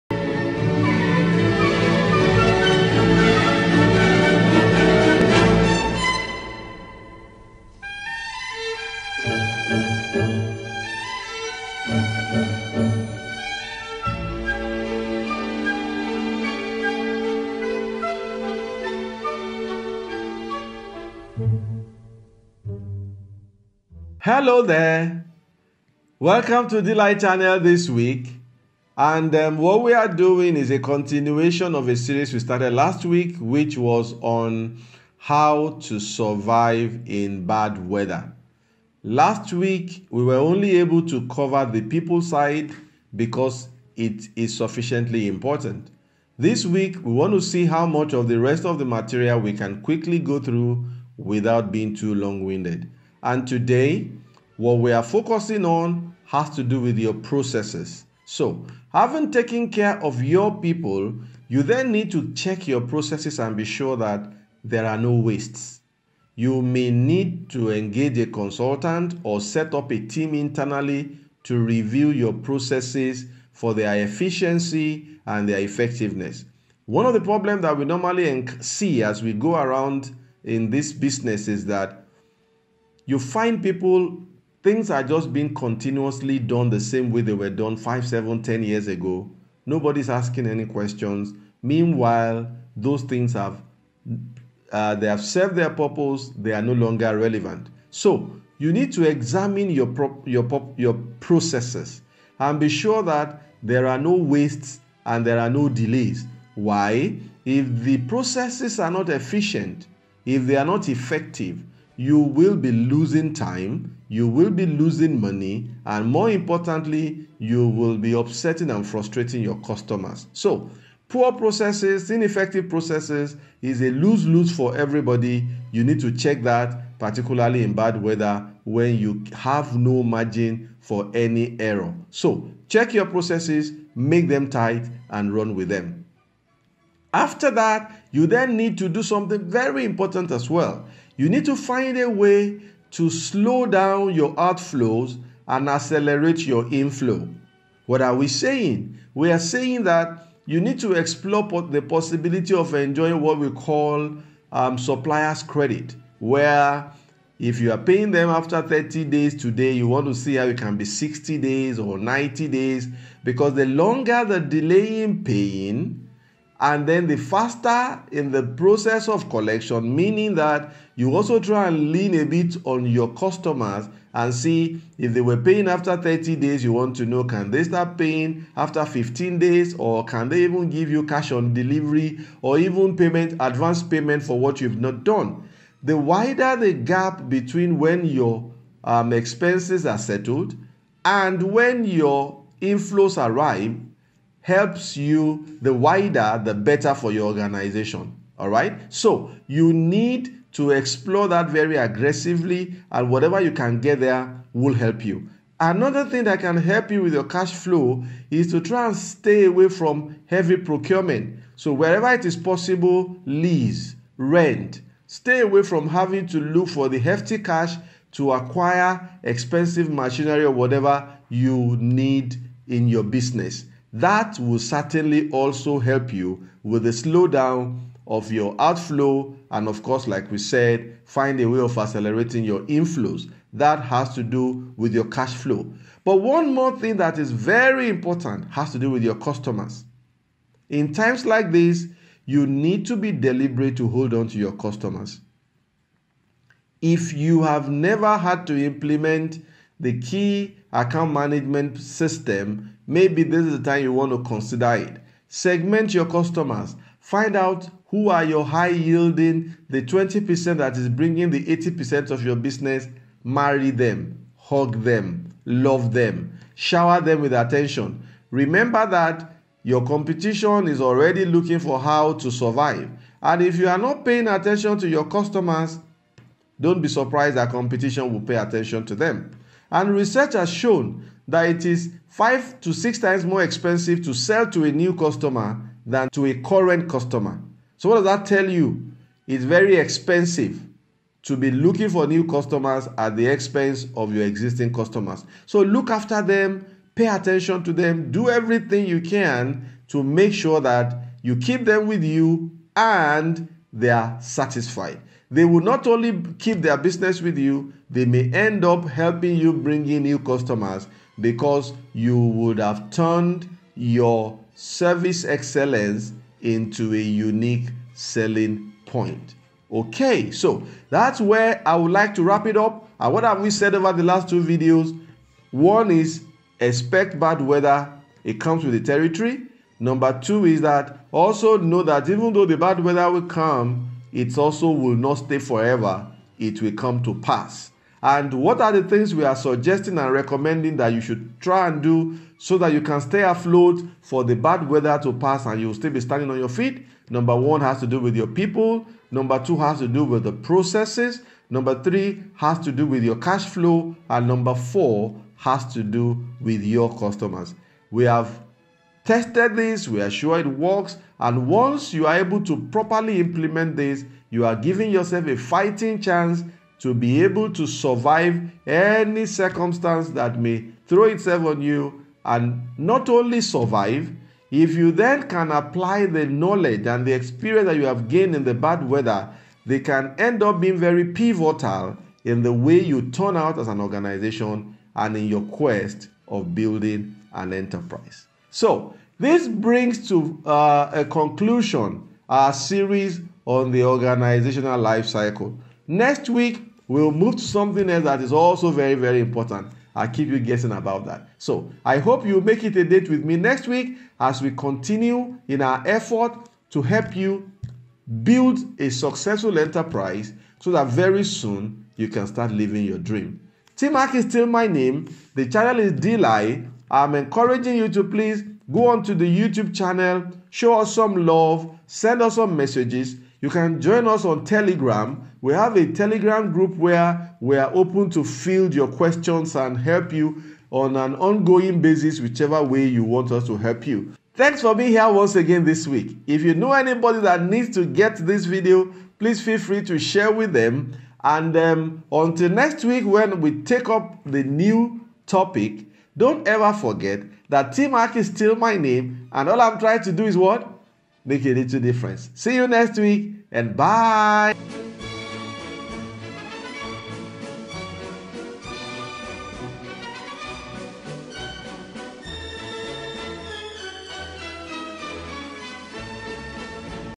Entrepreneurship: Surviving the Bad Weather - Other Factors
This week, we continue last week's conversation on how to survive the bad weather moments in Entrepreneurship